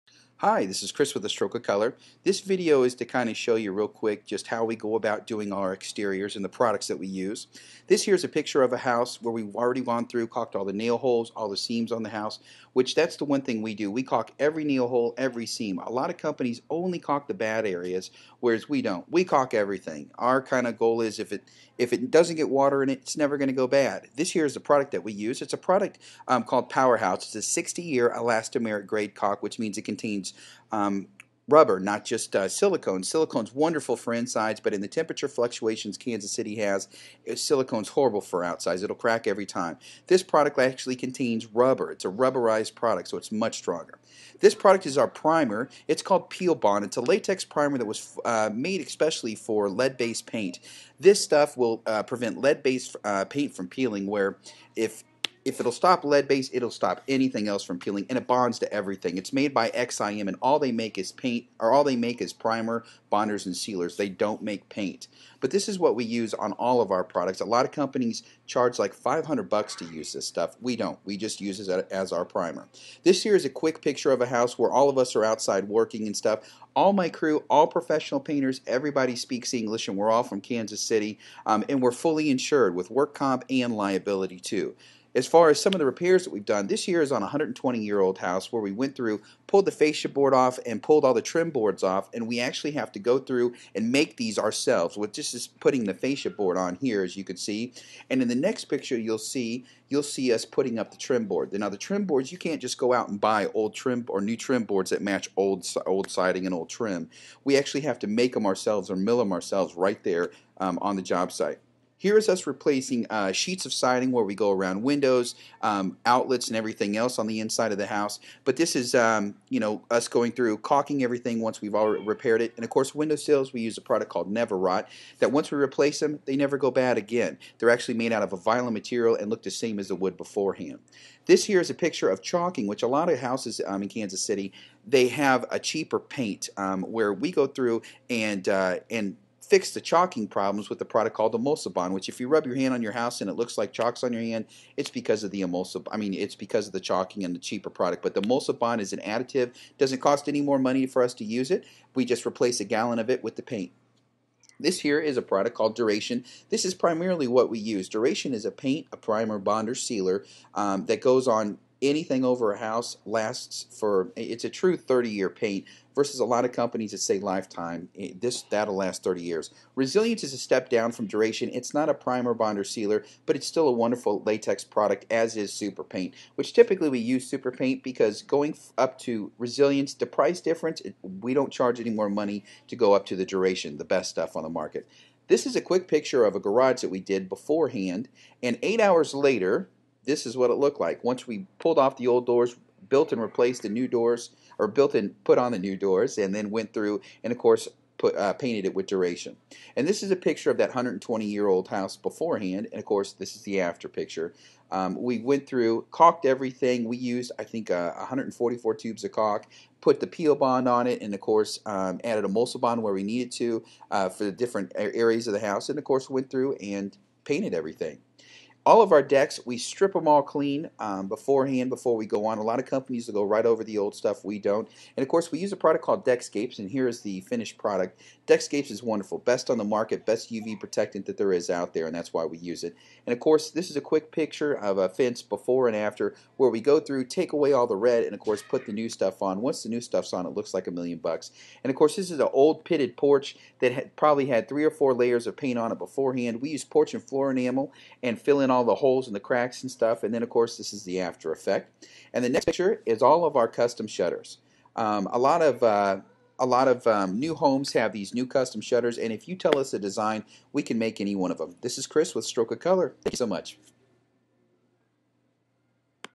A Stroke of Color Exterior Prep and Painting Video
How A Stroke of Color preps and paints exterior buildings and omes